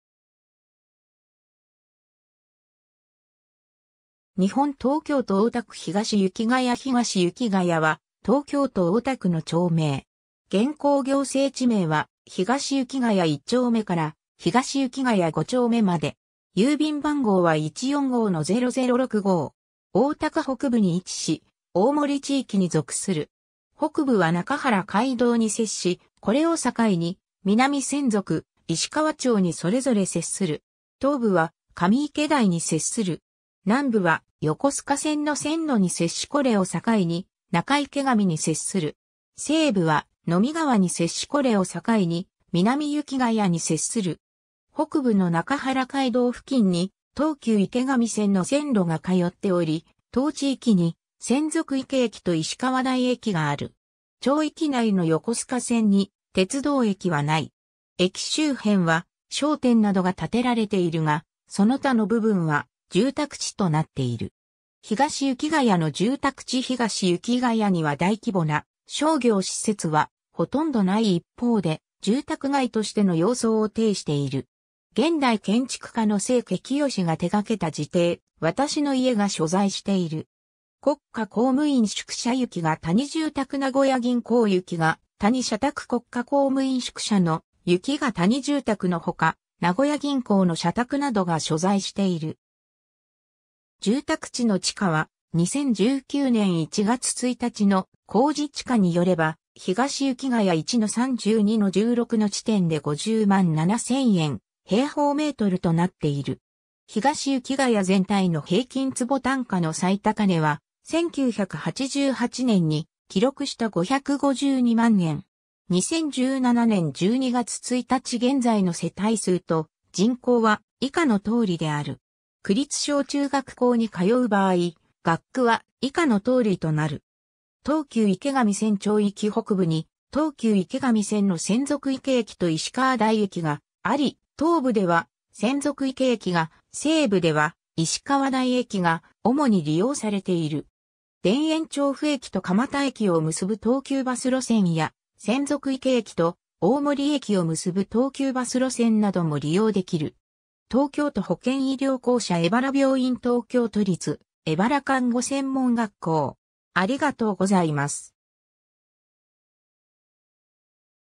東雪谷
日本 」 東京都 」 大田区 」 東雪谷 東雪谷（ひがしゆきがや）は、東京都大田区の町名。
現行行政地名は東雪谷一丁目から東雪谷五丁目まで。
 大田区北部に位置し、大森地域に属する。
北部は中原街道に接し、これを境に南千束・石川町にそれぞれ接する。
東部は上池台に接する。
南部は横須賀線（品鶴線）の線路に接しこれを境に、仲池上に接する。
西部は呑川に接しこれを境に南雪谷に接する。
（地名はいずれも大田区）北部の中原街道付近に東急池上線の線路が通っており、当地域に洗足池駅と石川台駅がある。
町域内の横須賀線に鉄道駅はない。
駅周辺は商店などが建てられているが、その他の部分は住宅地となっている。
 東雪谷の住宅地 東雪谷には大規模な商業施設はほとんどない一方で、住宅街としての様相を呈している。
現代建築家の清家清（息子は元慶應義塾長の清家篤）が手掛けた自邸「私の家」が所在している。
 国家公務員宿舎 雪ヶ谷住宅 名古屋銀行 雪ヶ谷社宅 国家公務員宿舎（官舎）の「雪ヶ谷住宅」のほか、名古屋銀行の社宅などが所在している。
 住宅地の地価は、2019年（平成31年）1月1日の公示地価によれば、東雪谷1-32-16の地点で50万7000円/m2となっている。
 東雪谷全体（3件）の平均坪単価の最高値は、1988年（昭和63年）に記録した552万円（バブル景気下）。
 2017年（平成29年）12月1日現在の世帯数と人口は以下の通りである。
 東急池上線 町域北部に東急池上線の洗足池駅と石川台駅があり、東部では洗足池駅が、西部では石川台駅が主に利用されている。
田園調布駅と蒲田駅を結ぶ東急バス路線や、洗足池駅と大森駅を結ぶ東急バス路線なども利用できる。
 東京都保健医療公社荏原病院 東京都立荏原看護専門学校